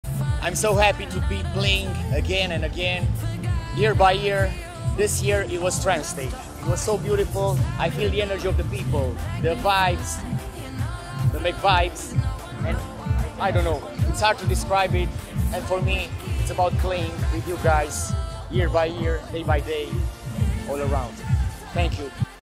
CDj Cristian-Daniel interview @untoldfestival Tram Stage
DJ, MC, foto& video, sonorizari si atmosfera de festival la evenimente corporate .

Interviu realizat de Iustin Grigoras la scena Mc'Donalds